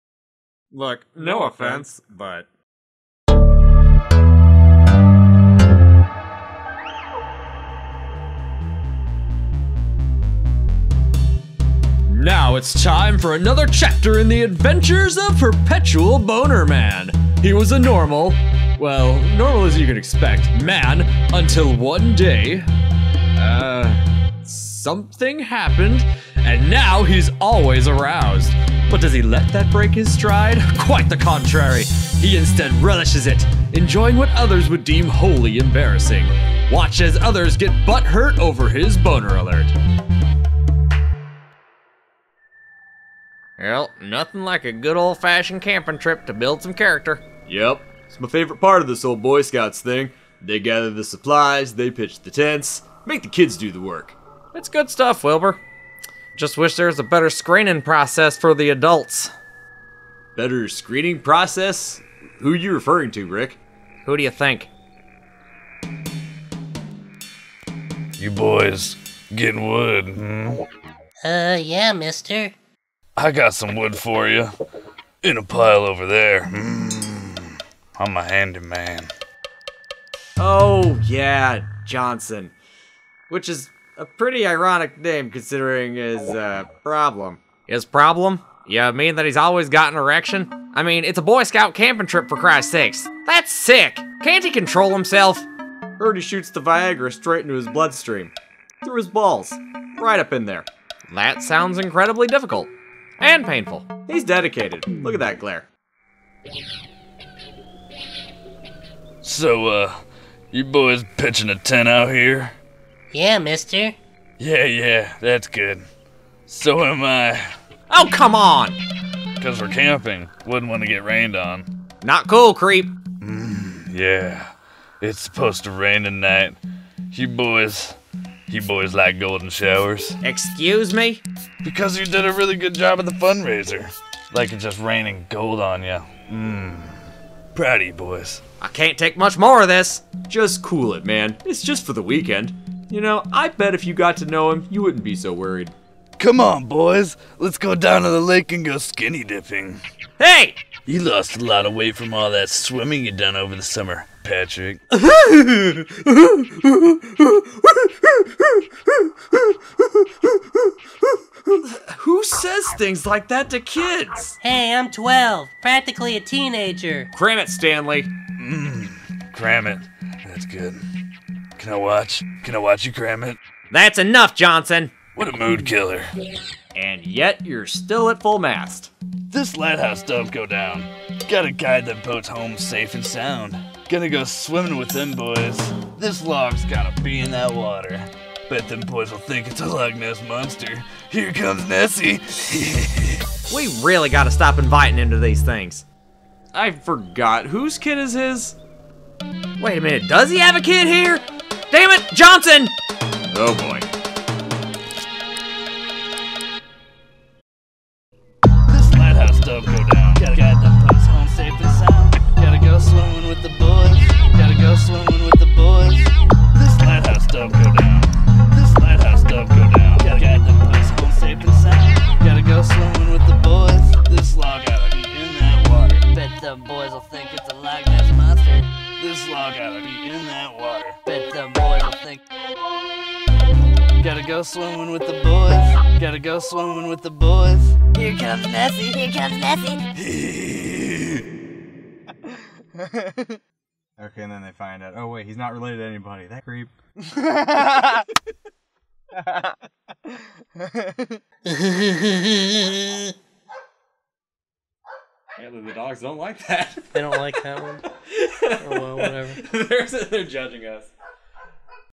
Pitch a Tent
A camping trip with Johnson gets awkward.